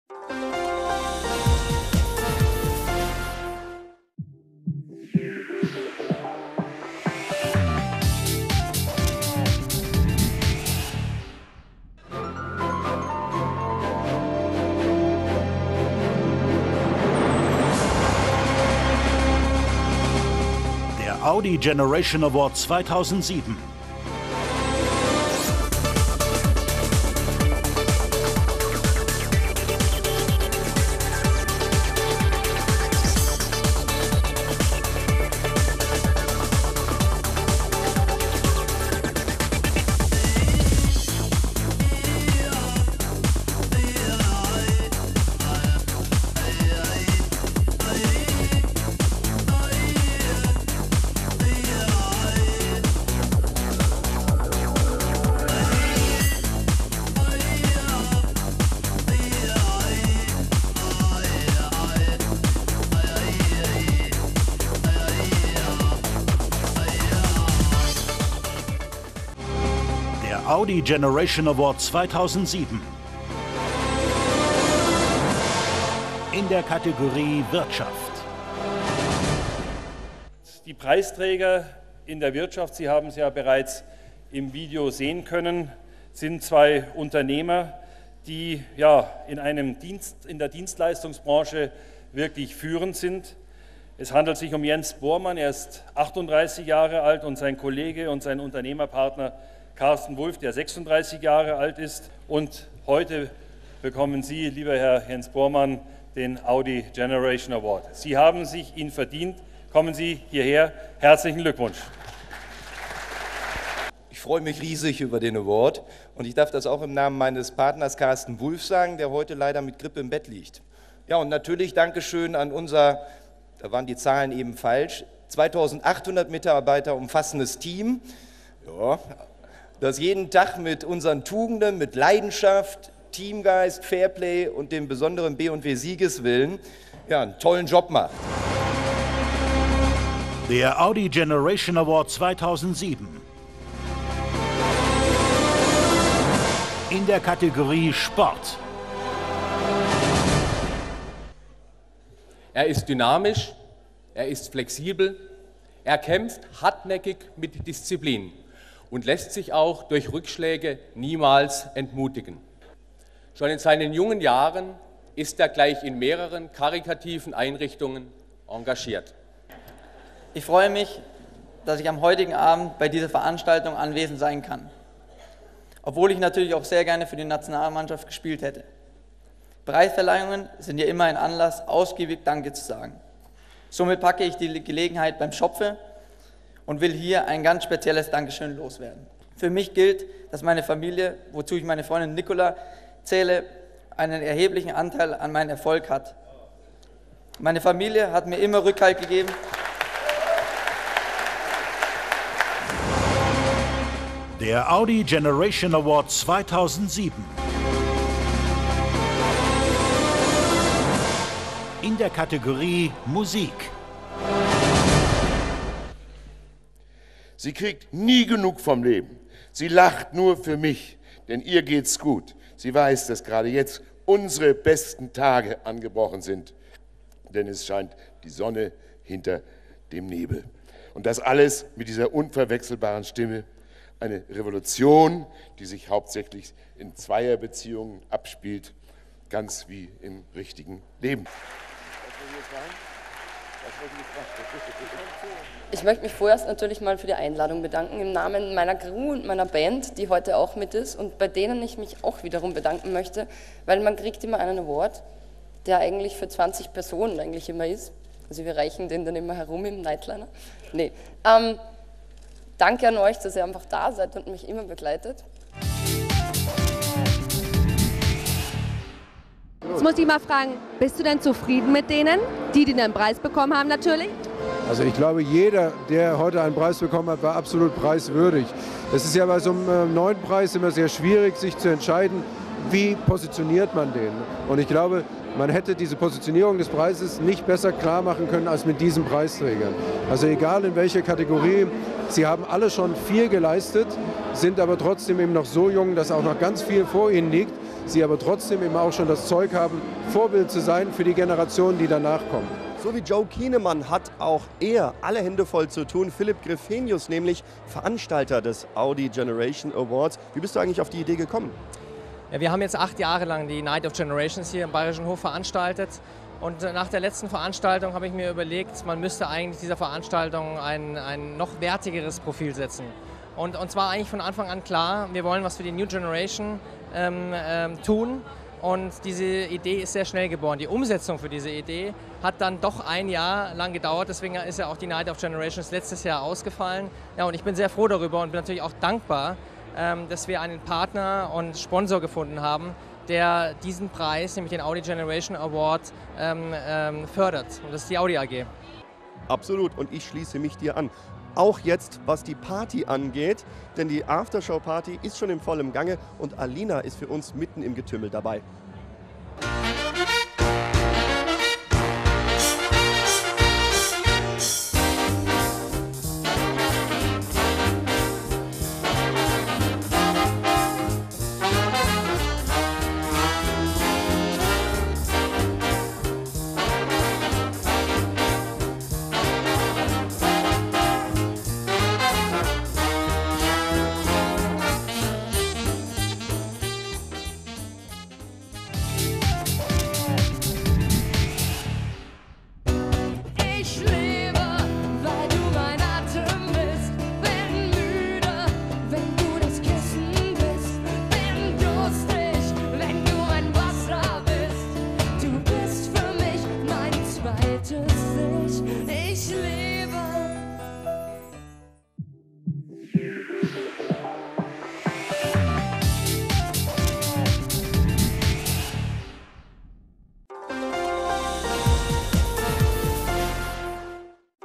Audi Generation Award 2007
Der von Philip Greffenius veranstaltete Audi Generation Award wird dieses Jahr erstmals am Samstag, den 13. Oktober 2007 im Rahmen eines Gala-Abends im Hotel Bayerischer Hof in den Kategorien Wirtschaft, Sport und Musik verliehen.
Diesen besonderen Award erhalten junge, bereits erfolgreiche Persönlichkeiten, die durch ihre bisherigen beruflichen Leistungen und dem damit verbundenen Wirken und Auftreten bereits etwas signifikantes, hervorragendes geleistet haben.

Die ersten Preisträger des Audi Generation Awards sind Fussball-Nationalspieler Philipp Lahm, die österreichische Rocksängerin Christina Stürmer und die beiden Geschäftsführer der „buw"-Unternehmensgruppe Jens Bormann und Karsten Wulf. Überreicht werden die Preise von den Laudatoren Ralph Weyler, Vorstand Marketing und Vertrieb der Audi AG für die Kategorie „Wirtschaft", Herbert Hainer, Vorstandsvorsitzender der adidas AG für die Kategorie „Sport" und Prof. Christian Bruhn, Aufsichtsratsvorsitzender der GEMA für die Kategorie „Musik".

Der von Kai Pflaume moderierte Preis, wird im Rahmen einer Gala vergeben, die jung und alt aus den Bereichen Wirtschaft, Medien, Sport und Gesellschaft für eine glanzvolle Nacht zusammenbringt. Die Atmosphäre der Gala „Nacht der Generationen", zu der Philip Greffenius bereits achtmal eingeladen hat, wird mit ihrem unverwechselbaren Ambiente im Ballsaal des Hotels Bayerischer Hof nach einjähriger Pause wieder aufleben und mindestens drei Generationen von interessanten Gästen zusammenführen, um die Preisträger zu feiern.

Viel Spass wünscht Philip Greffenius von Edition Sportiva!